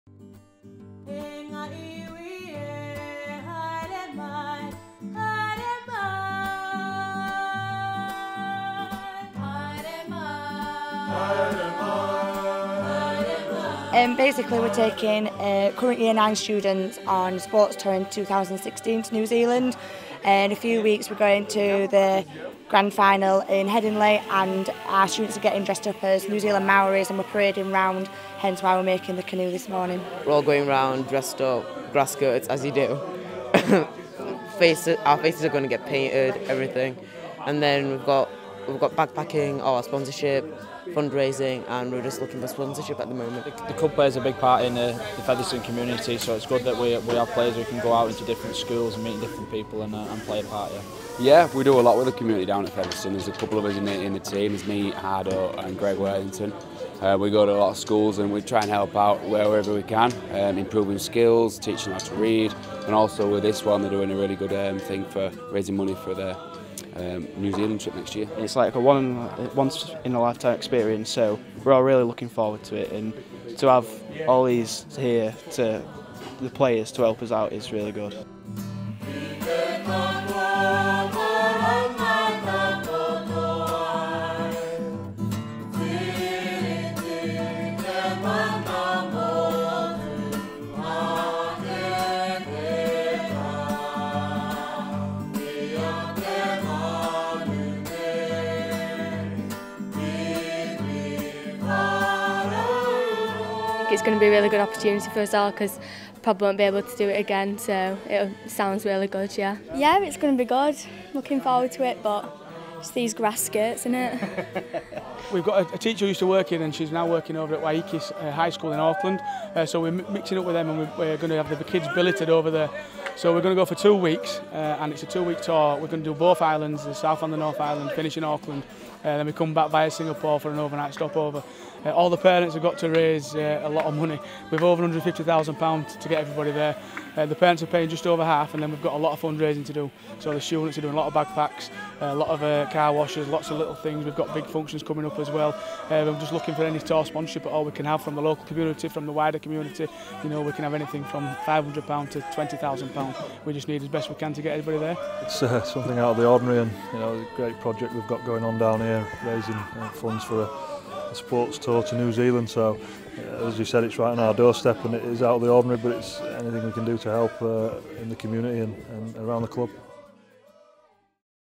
Featherstone school heading for New Zealand
St Wilfrid's Catholic High School are aiming to send their sports teams on a once-in-a-lifetime trip to New Zealand - with a little help from Featherstone Rovers